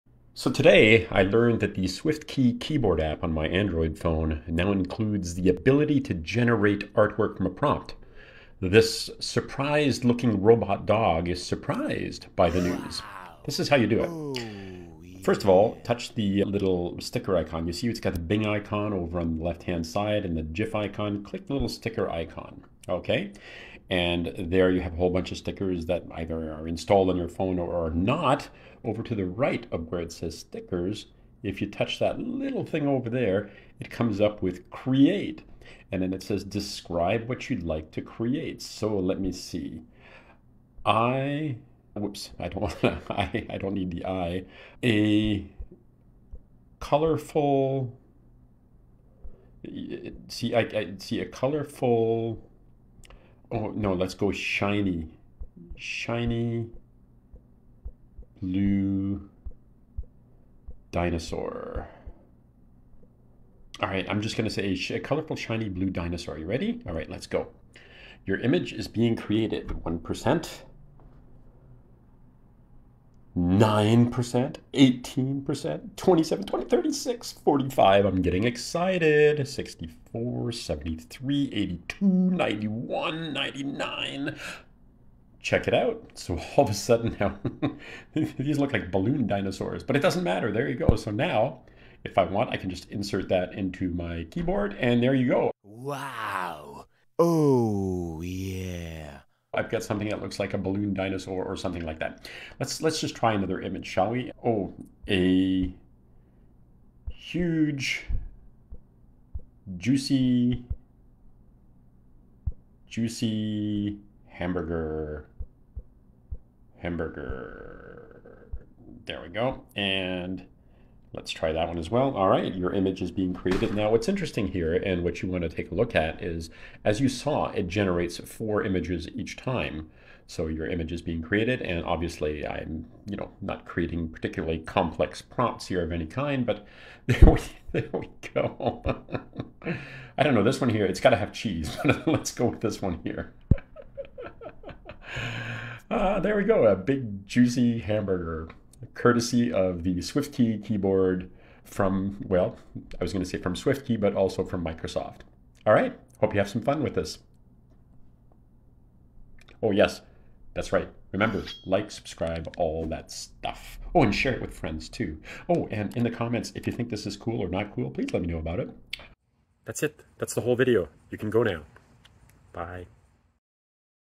Create Your Own Ai Images Using Swiftkey!
A new update from SwiftKey, my goto keyboard app for Android, now includes generative AI for images. You can now create your own artwork, on the fly, from your keyboard. Let me show you how it's done!